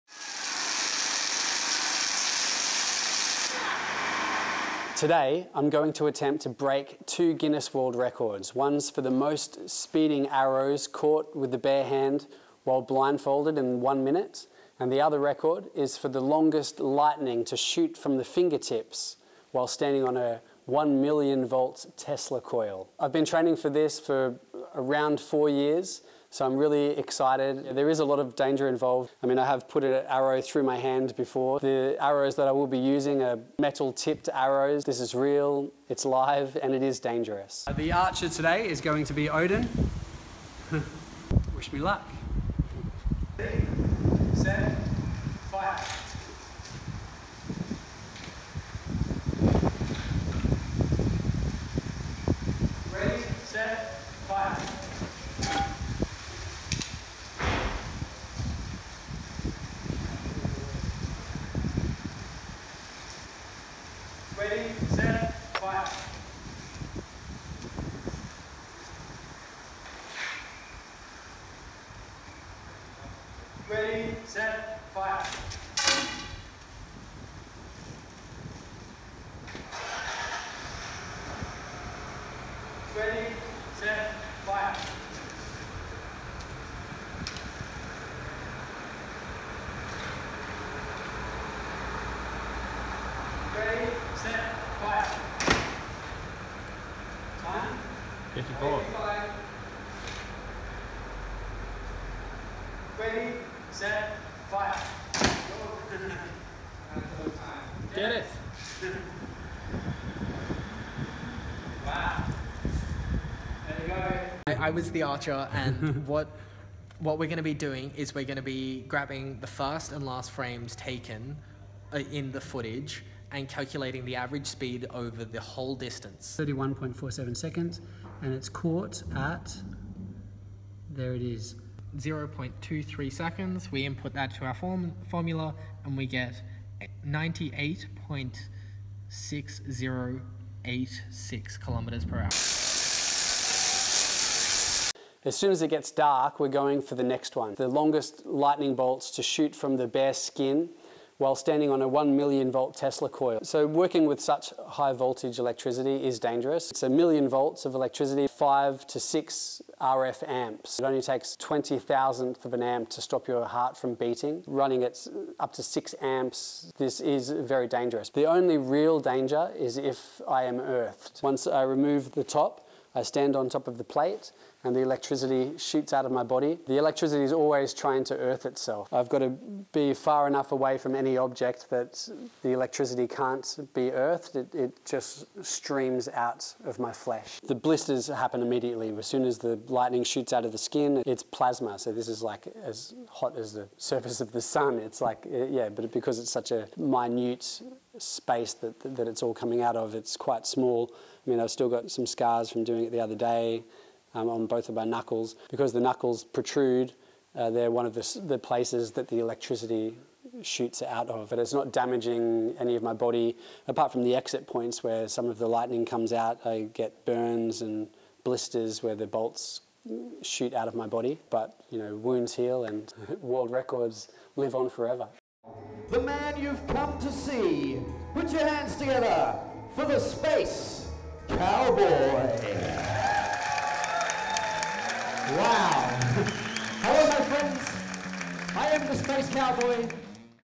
The Space Cowboy | Guinness World Records Day 2018 | Blind Arrow Catch | Longest Spark
On 8th November 2018, Chayne Hultgren "The Space Cowboy" made two new Guinness World Records attempts in Byron Bay, Australia.

Chayne Hultgren is Australia's most prolific Guinness World Records breaker. On this occasion he managed to create the most arrows caught while blindfolded in a time of 1 minute as well as the longest spark to come out of his own bare flesh!

Directed, Filmed and Edited by David Peace One Love for an film making assignment in accord with SAE QANTM Creative Media Institute, overseen by executive producer Travis Hanley.